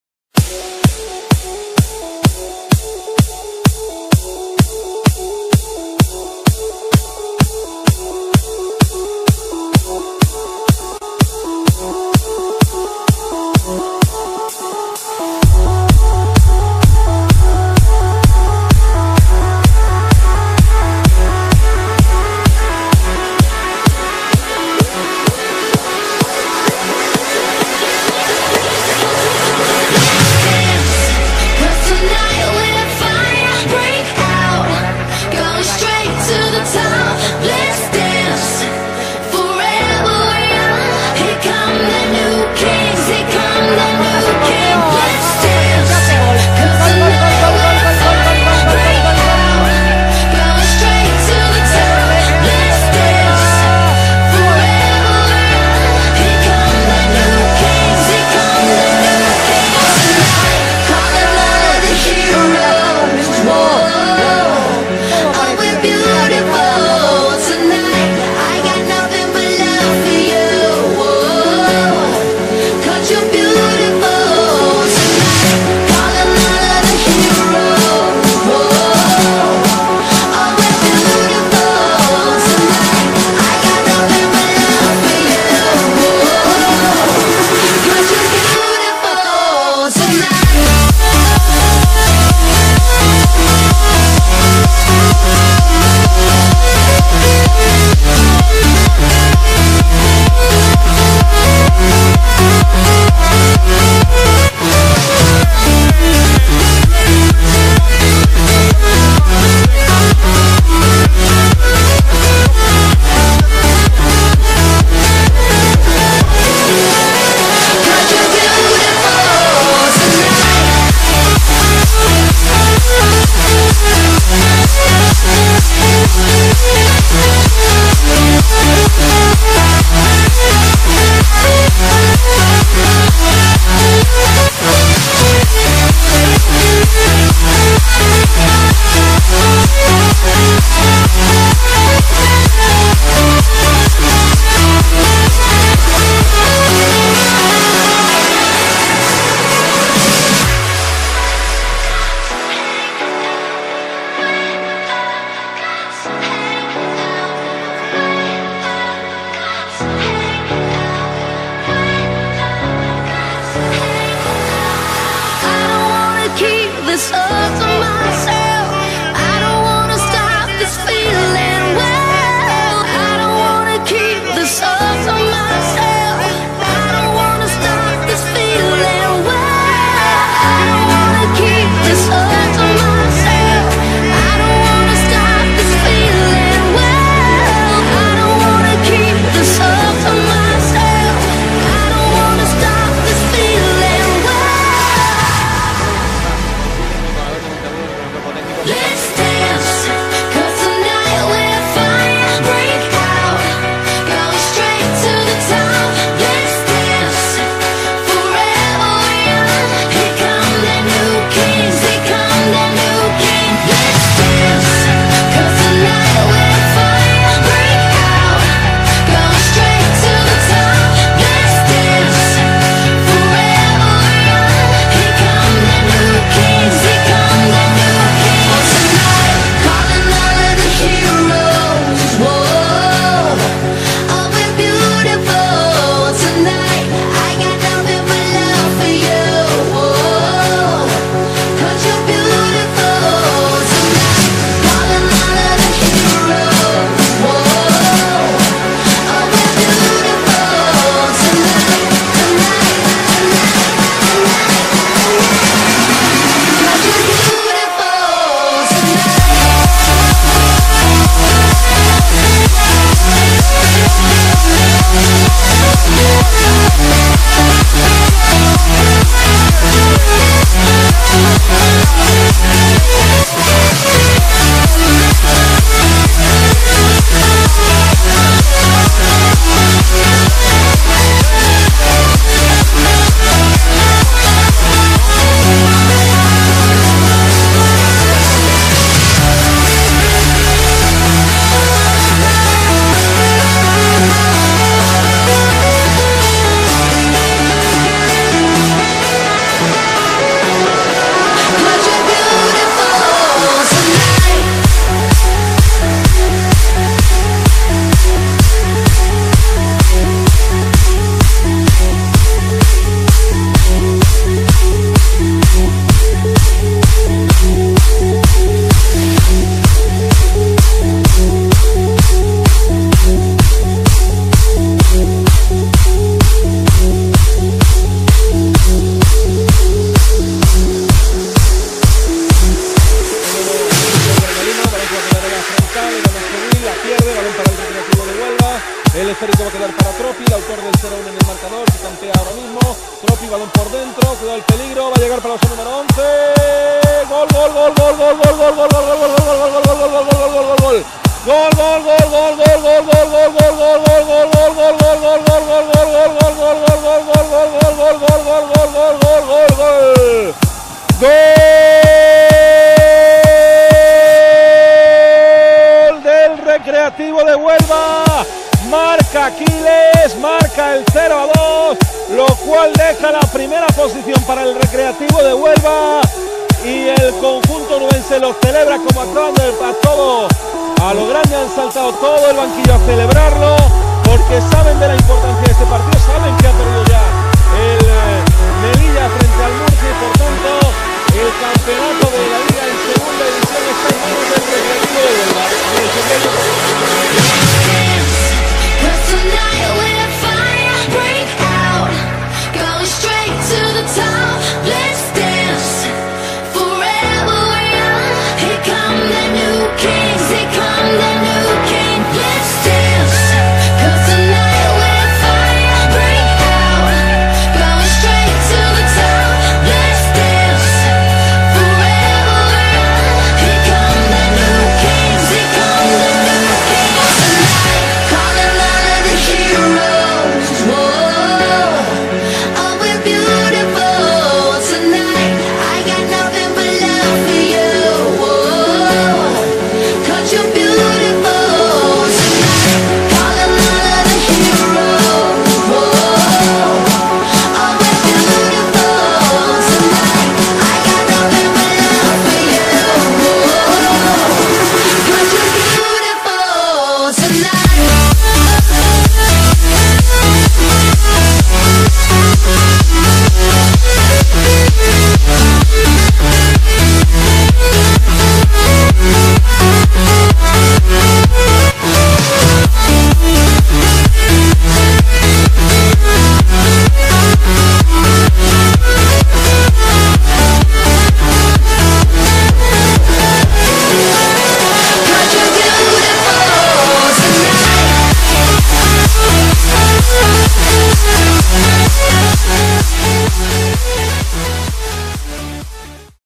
Alberto Quiles TOP GOLES | Recreativo de Huelva
¡Comenta cual es tu gol favorito de nuestro máximo goleador en esta temporada!

🗝️ ¿Compras en Instant Gaming? Utiliza este enlace a la hora de hacer una compra para ayudarme:

🎮 Club de eSports: Recreativo Thunder (sección oficial de eSports del Real Club Recreativo de Huelva)⚡